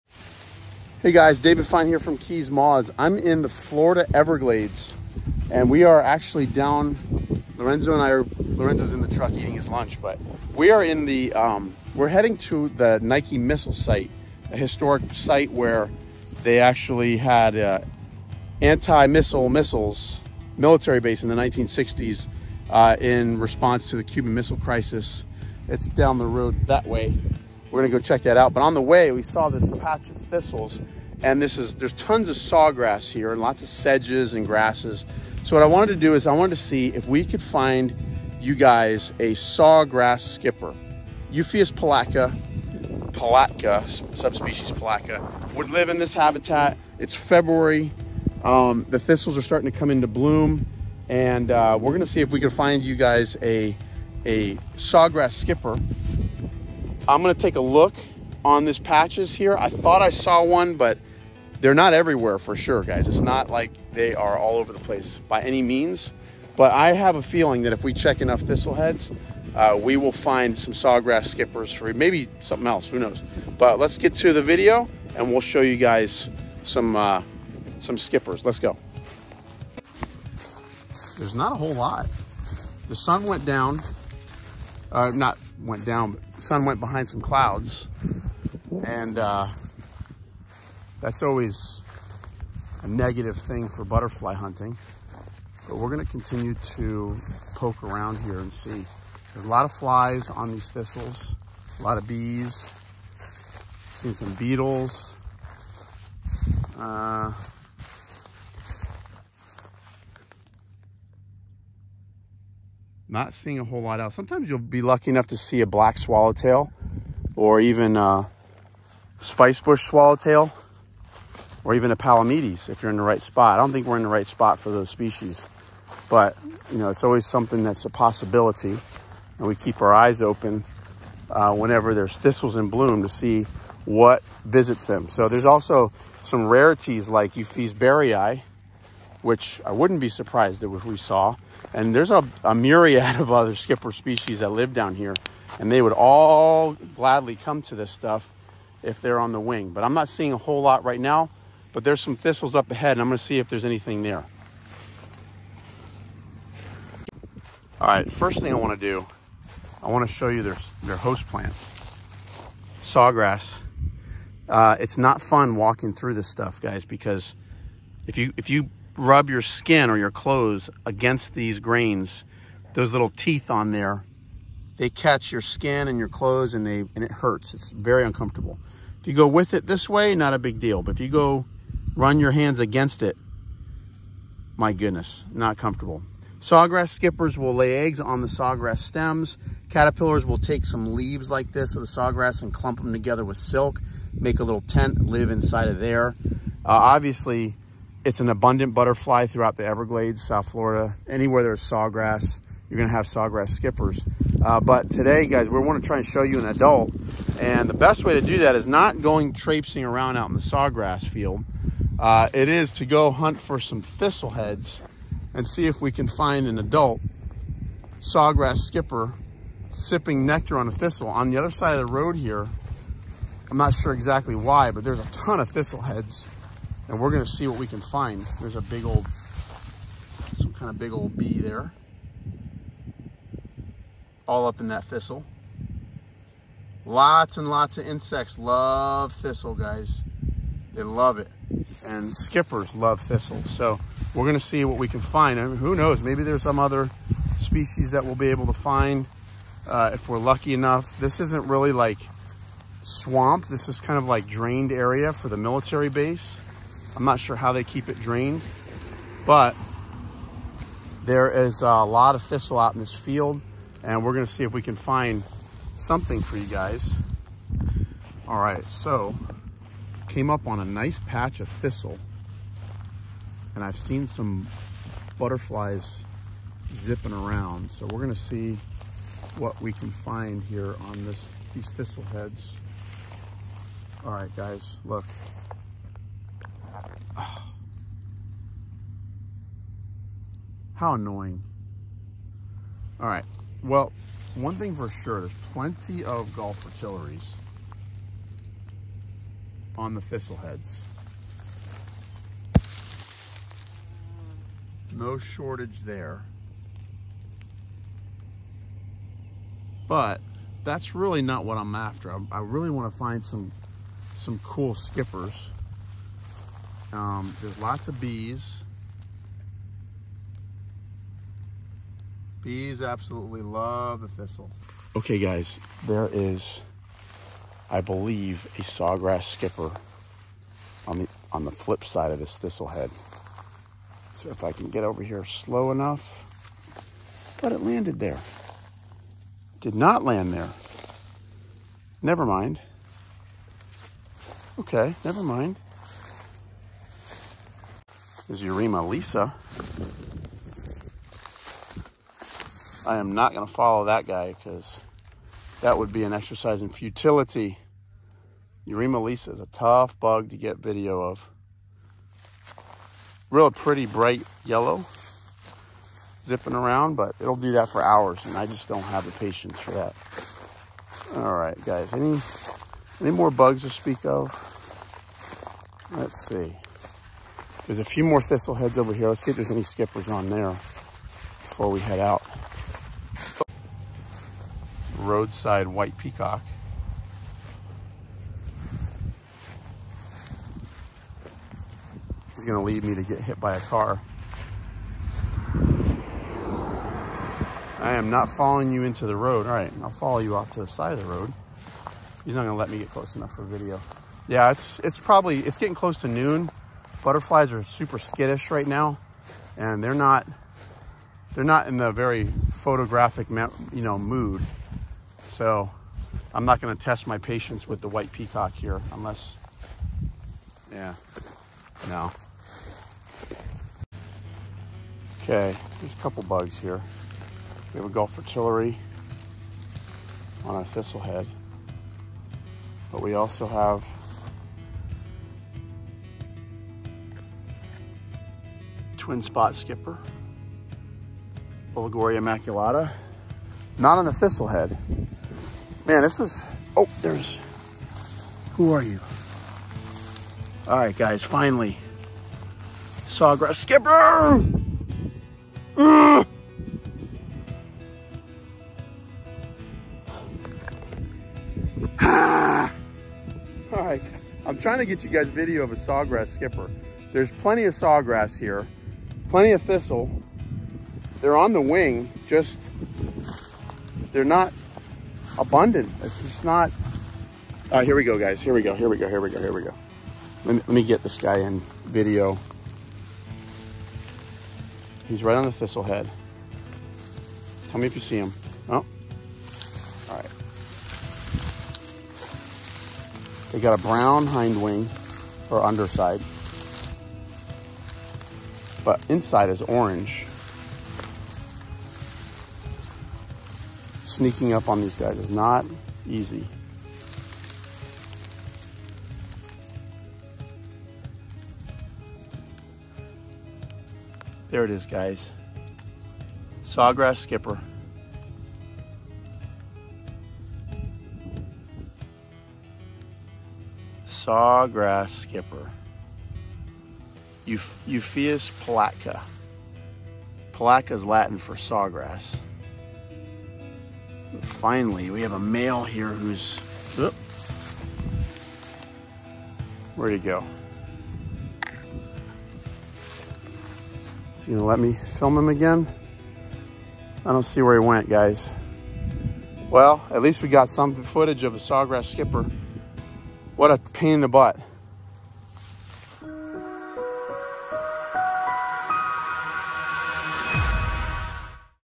THIS SKIPPER BUTTERFLY EATS SAWGRASS!!! ONE TOUGH BUG
This is one tough butterfly. The sawgrass skipper lives in the swampy habitats of south Florida and has a very peculiar larval host plant. The caterpillars eat sawgrass leaves. If you have ever touched a sawgrass blade, you would find it challenging to think about actually eating it. These little caterpillars love it! In this video, we will be in Everglades National Wildlife Refuge looking for this elusive skipper butterfly and will show you an easy way to find them!